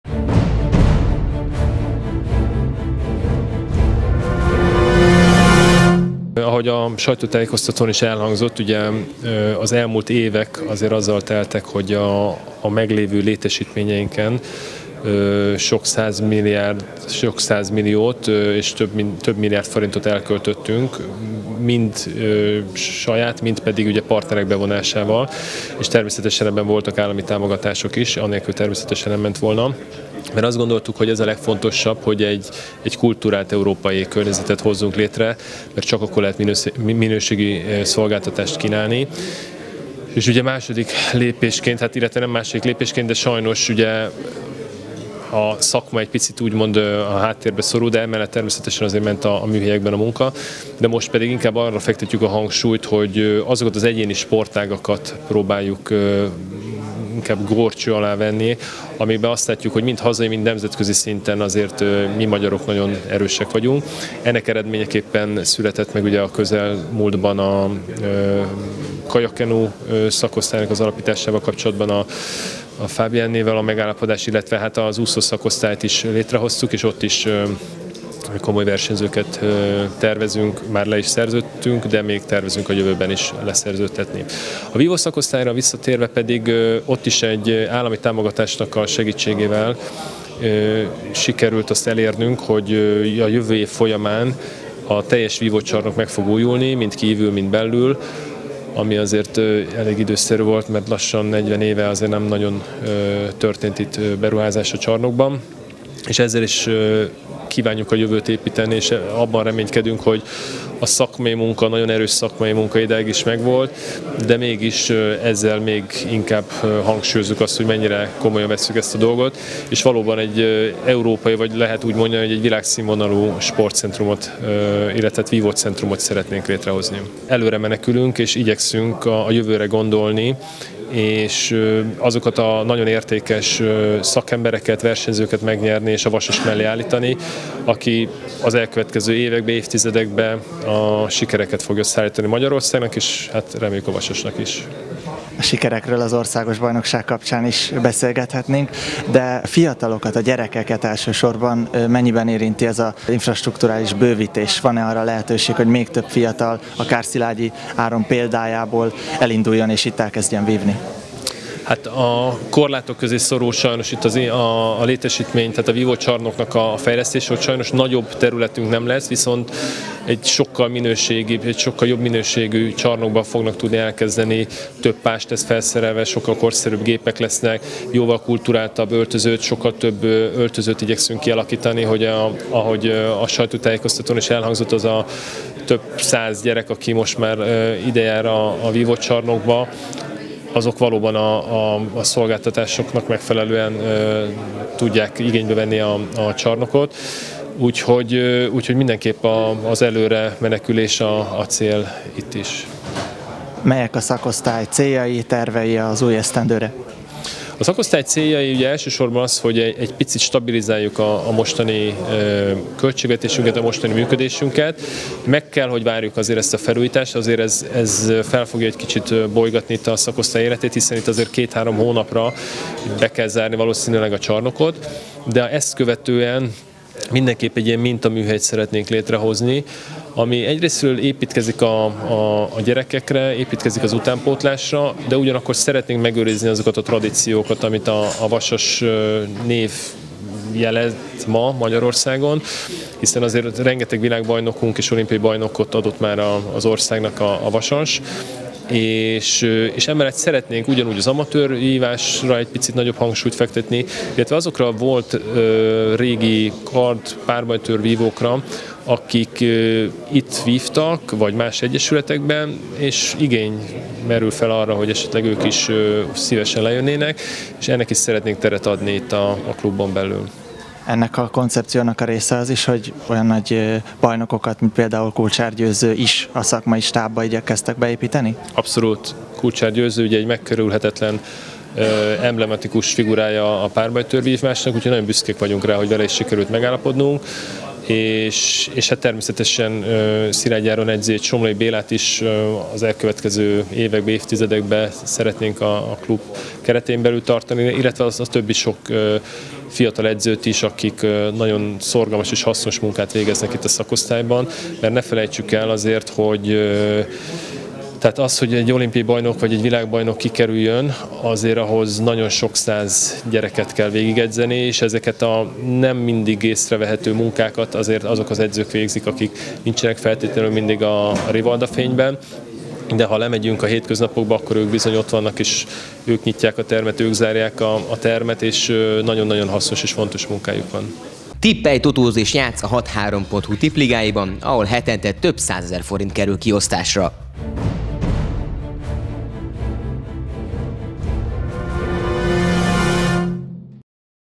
Világszínvonalú vívócentrumot szeretnénk létrehozni - Ferjancsik Domonkos
Komoly tervekkel készül az új esztendőre az idén 101 éves Vasas Sport Club, amely világszínvonalú vívócentrum létrehozására törekszik. Az 1911-ben alapított patinás egyesület tavasszal egy minden igényt kielégítő csarnok építésébe kezd a vívószakosztály számára, a szakmai munkában pedig az utánpótlás nevelés és a tradíció megőrzésének párhuzamát kívánja erősíteni. Az infrastrukturális fejlesztésekről, az állami szerepvállalás fontosságáról, valamint a klub ikonjainak tevékenységéről Ferjancsik Domonkos, a vívószakosztály elnöke beszélt a Hathárom TV kamerája előtt.